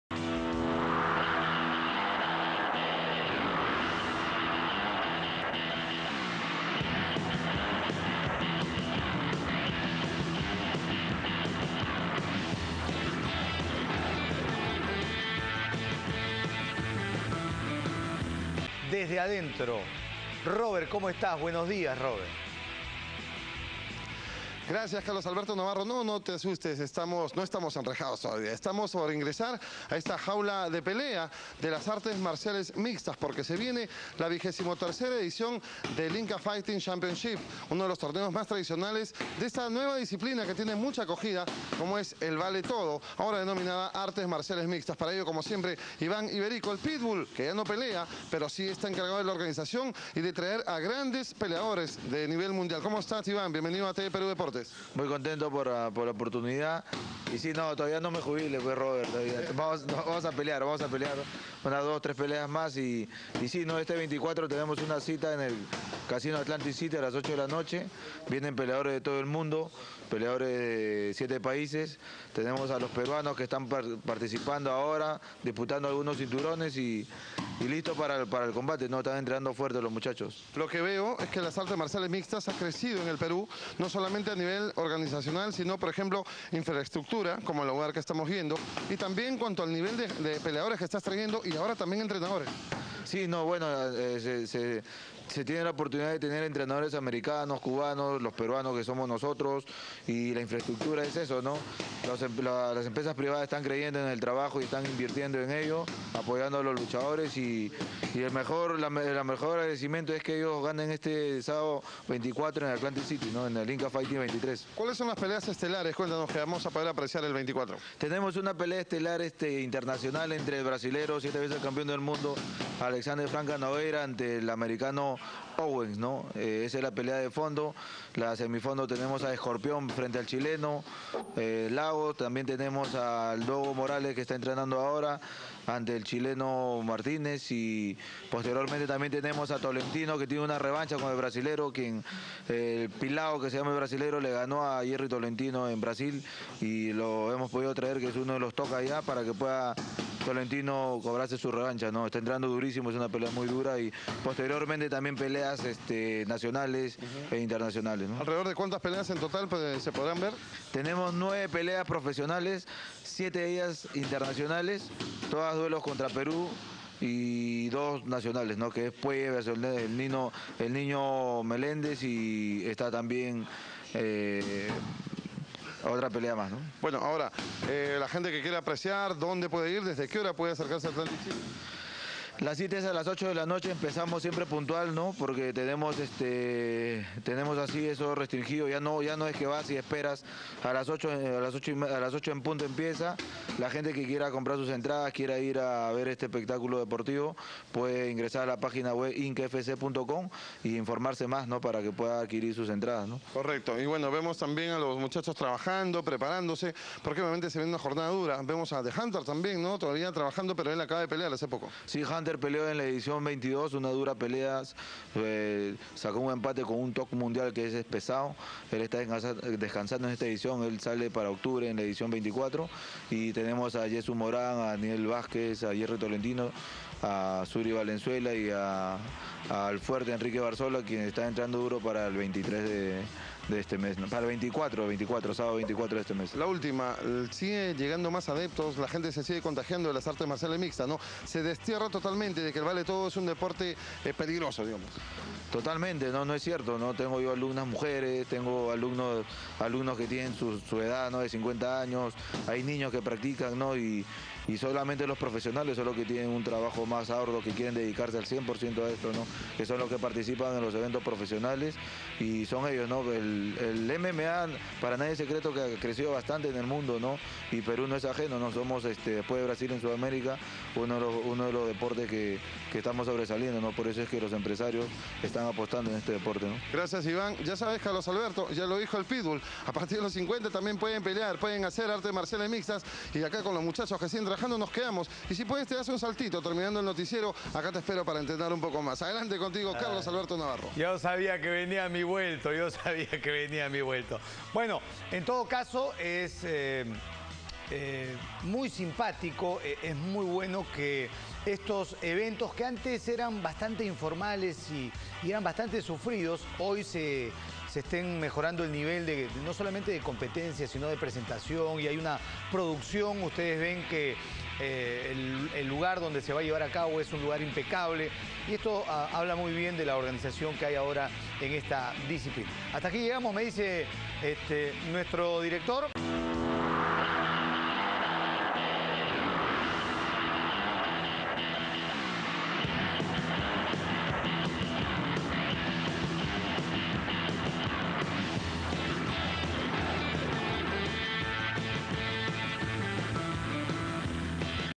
Promocion del IFC 23 por el canal 7 (Tv Peru Deportes)
Esta entrevista se realizo el 9/08/2013 por Robert Malca quien es fanatico de las mma.